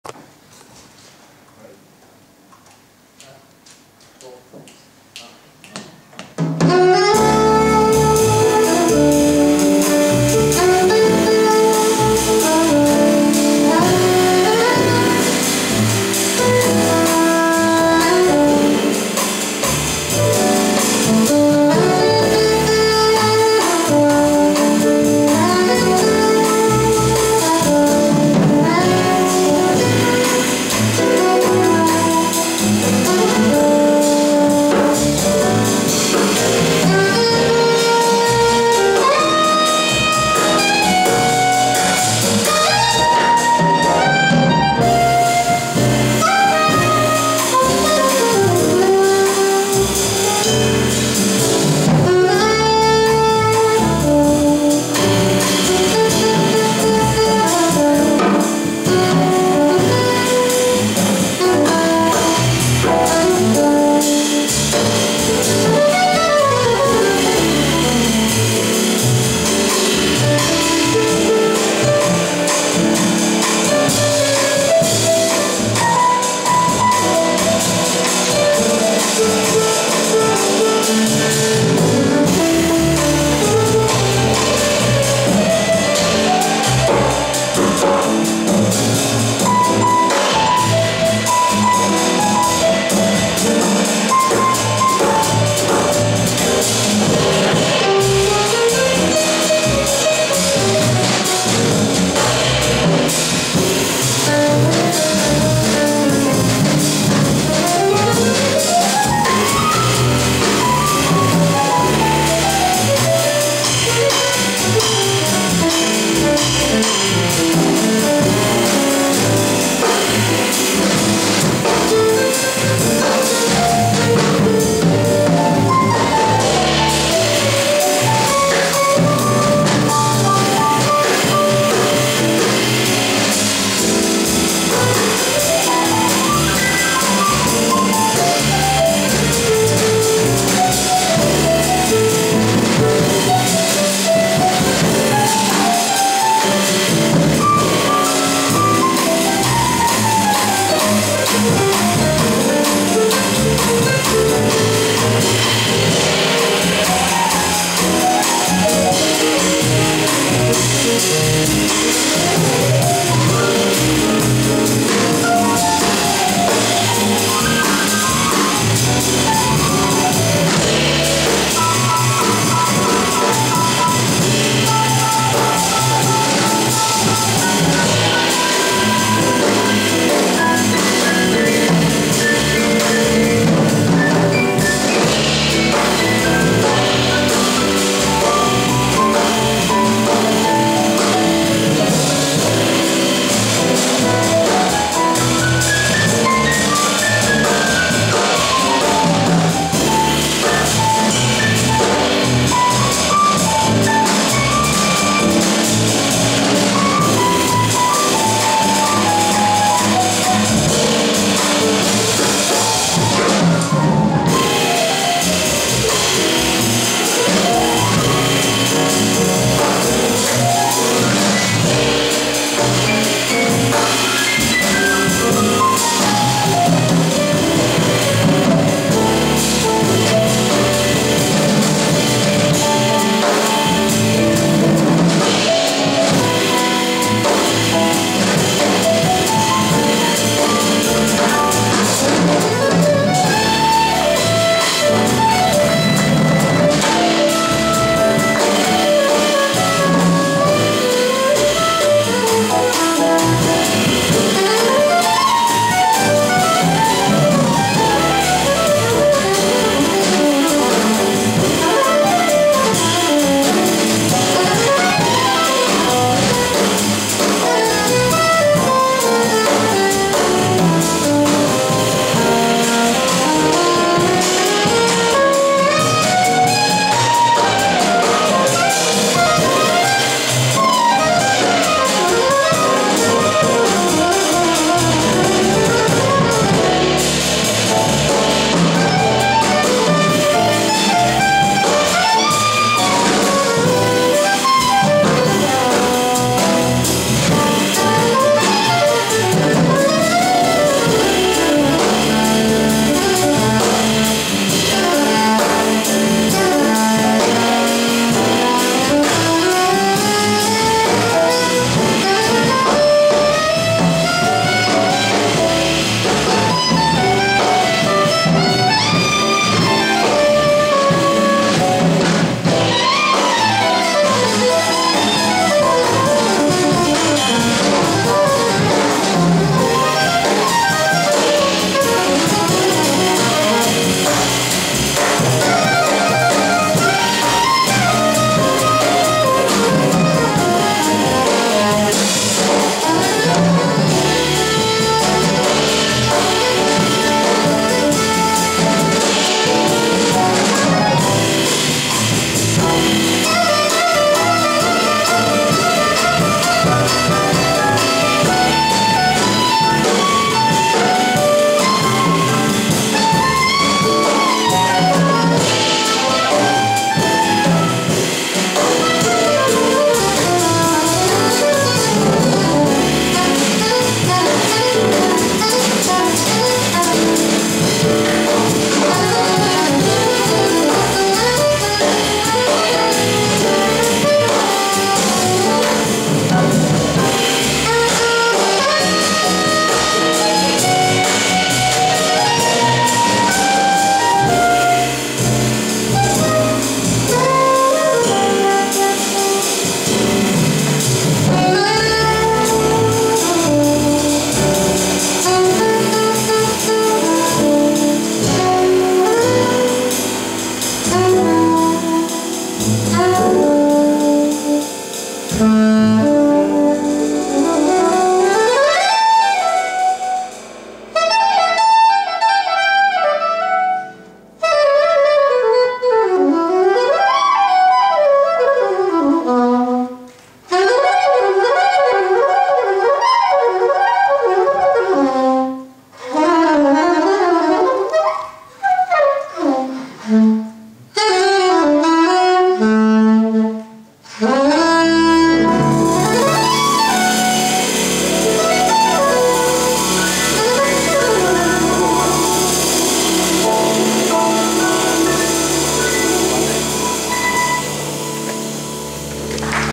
Karl-Henrik Ousbäck-Håkan Broström kvartett 30/3 2019
Spelning på Restaurang Skepparpinan i Motala Folkets Hus. Arrangör Motala Jazz & Bluesklubb
Karl-Henrik Ousbäck, trummor
Håkan Broström, sopran och tenorsax
Leo Lindberg, piano
Arvid Jullander, bas